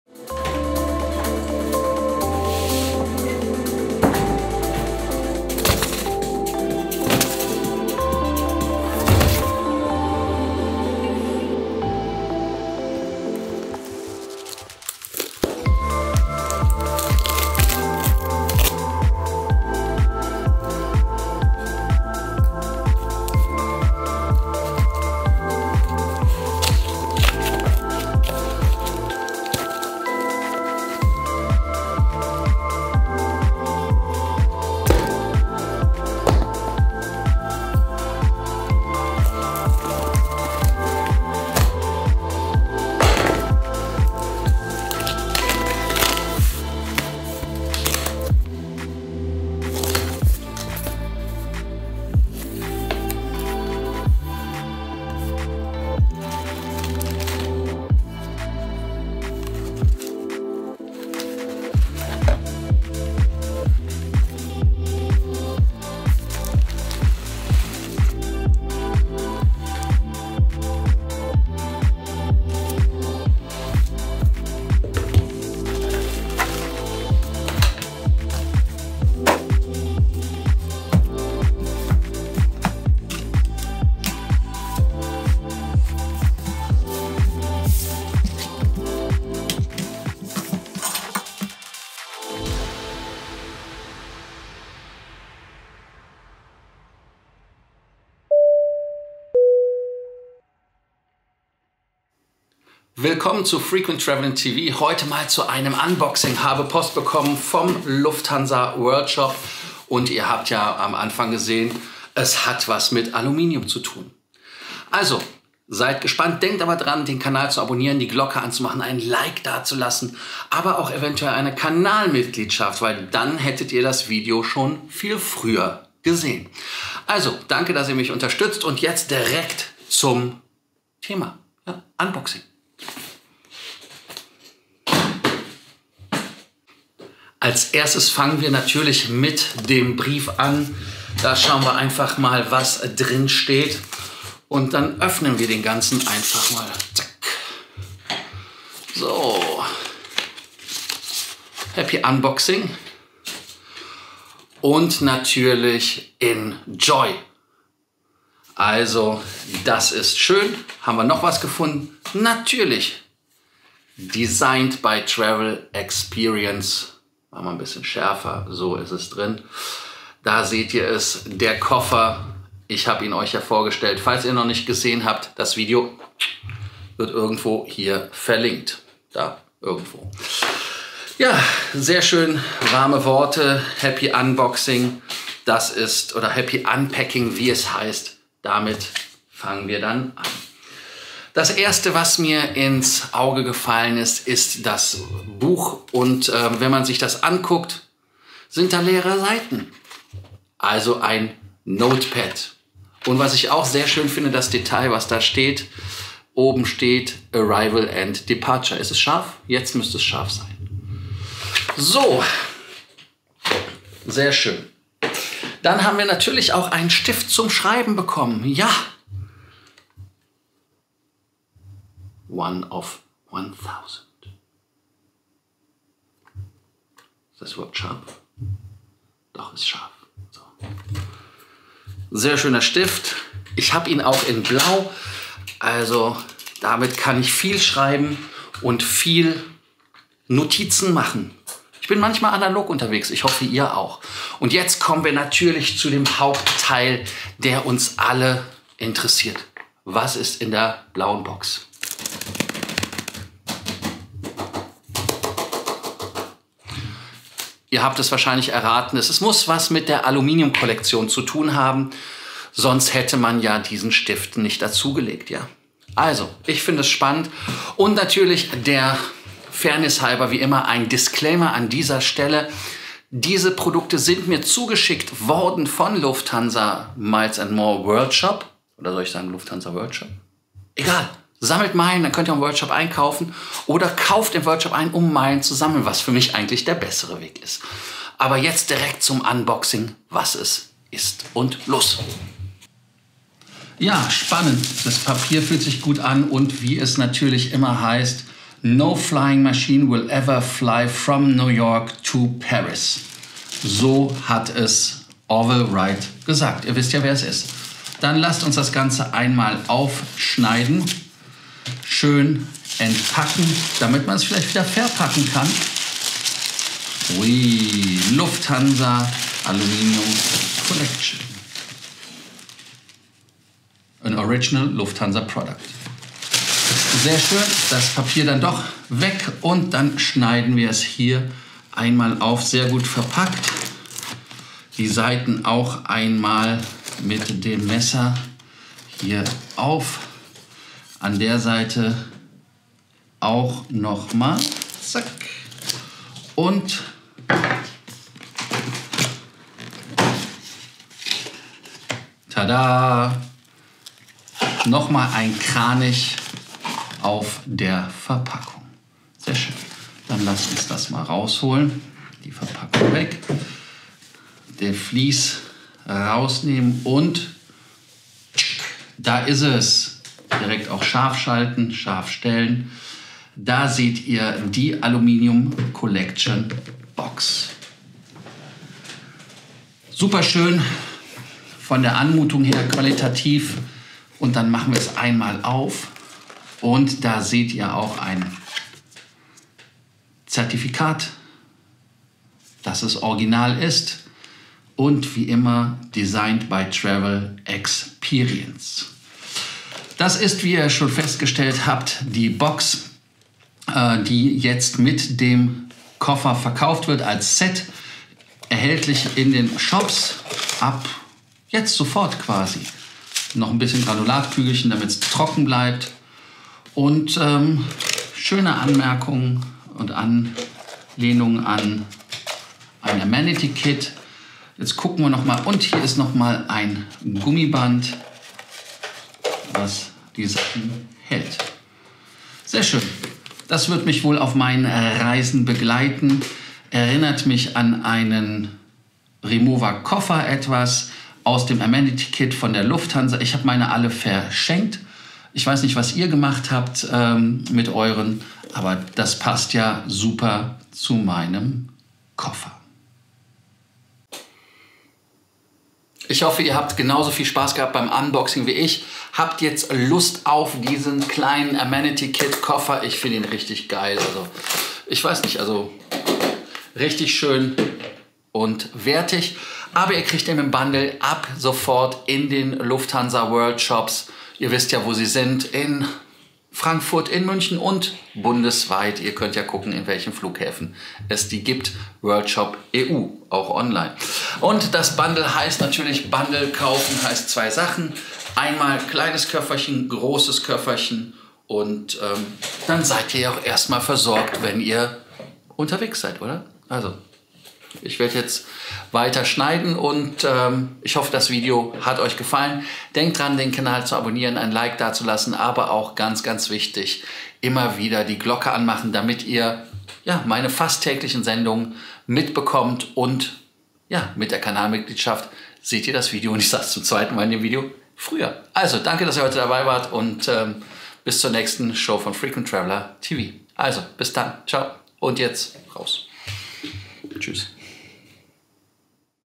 Lufthansa Aluminium Collection Amenity Kit unboxing
Lufthansa Aluminium Collection ist seit einigen Wochen erfolgreich auf dem Markt. Nun gibt es das Bundle. Bordtrolley und das Lufthansa Aluminium Collection Amenity Kit .

Wer den Koffer schon hat, kann mit der Rechnung in jedem Worldshop dieses Amenity Kit für 69 Euro kaufen.

Clubhouse - @LFCorsten